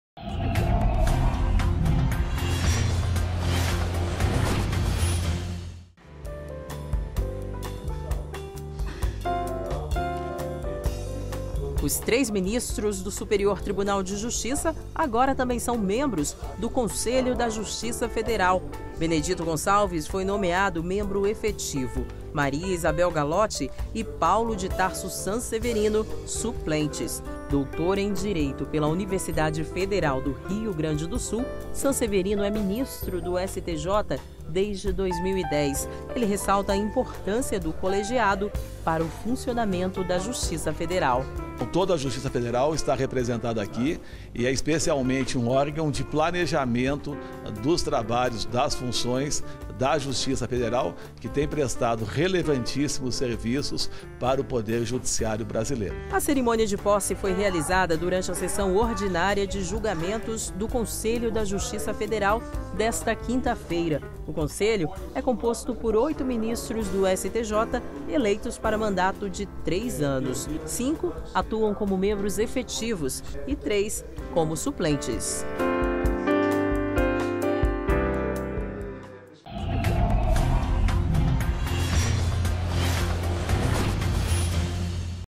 Vem Comigo- Posse Ministros CJF
Os ministros Benedito Gonçalves, Paulo de Tarso Sanseverino e Maria Isabel Galloti tomam posse como membros do Conselho da Justiça Federal.

Vem comigo!